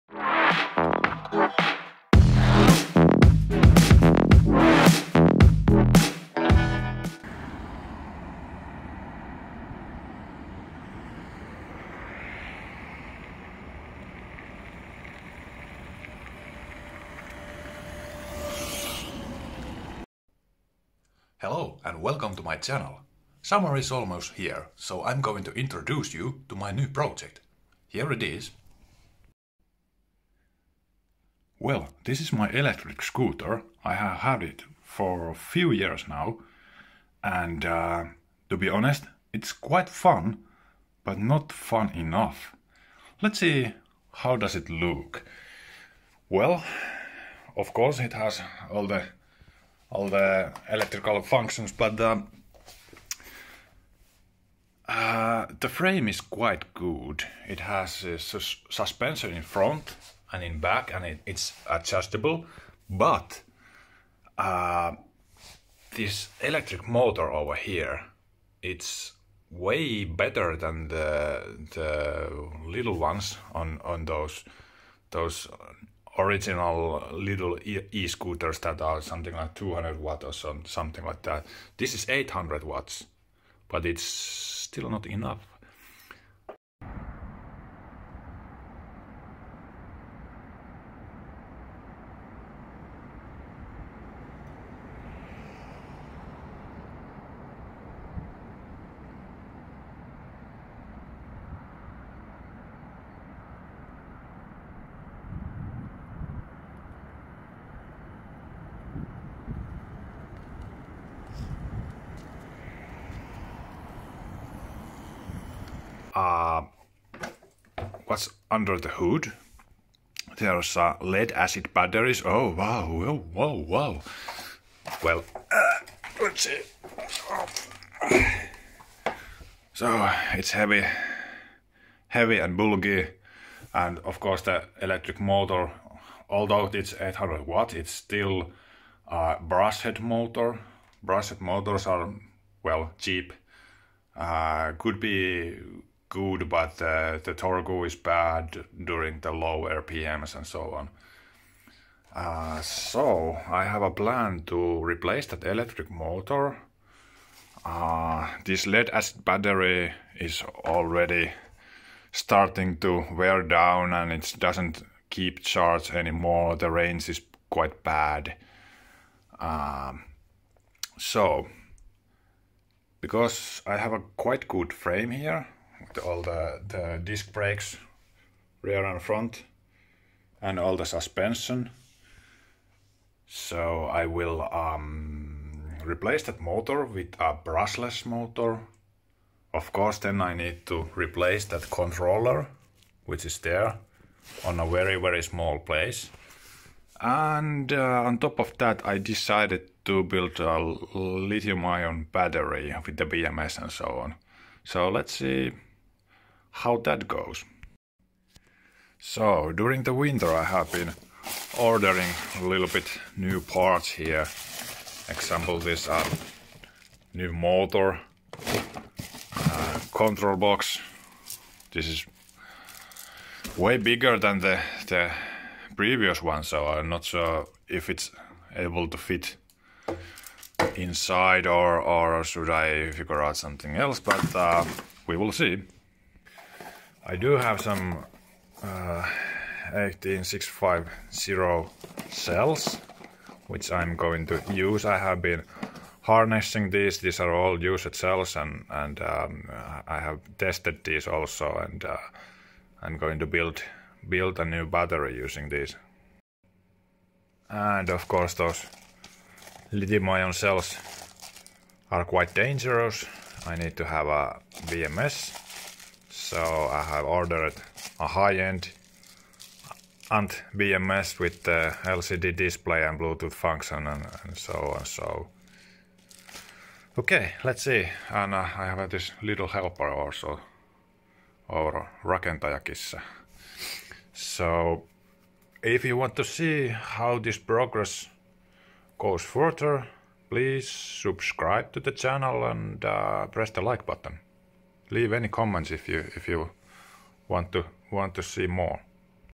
Introducing my new DIY project! E-scooter upgrade.
I'm going to start small video series around my new project. There will be tutorial how to change brushed motor to brushless one (including control box) and how to build a big lithium-ion battery pack for an electric scooter.